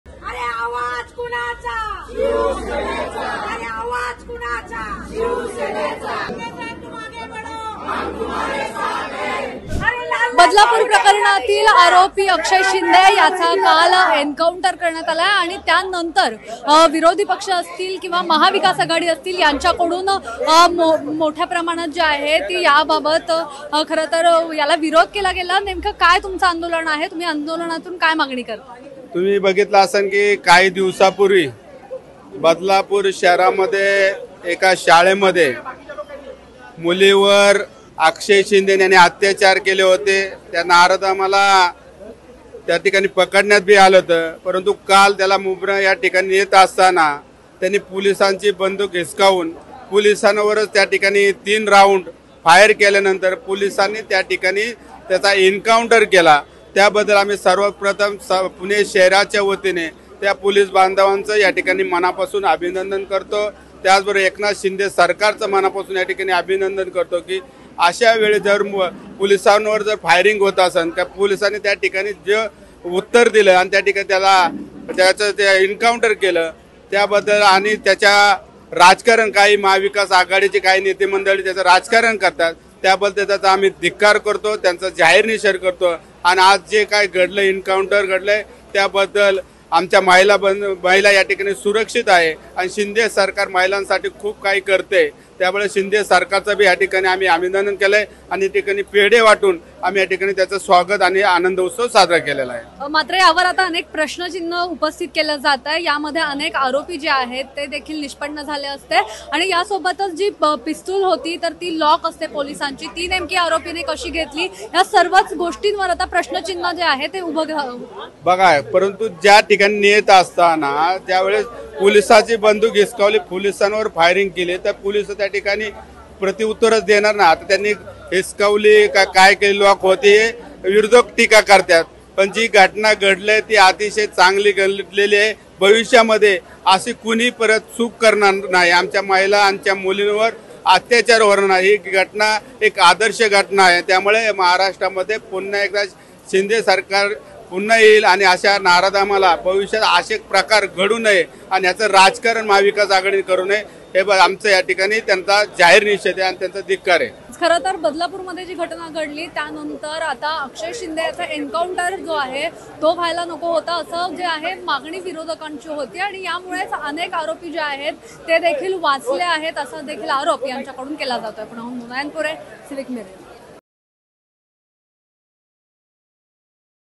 अक्षय शिंदे एन्काऊंटर प्रकरणात महाविकास आघाडीच्या विरोधात शिवसेनेचं आंदोलन | Civic Mirror
अक्षय शिंदे एन्काऊंटर प्रकरणात महाविकास आघाडीच्या विरोधात शिवसेनेचं आंदोलन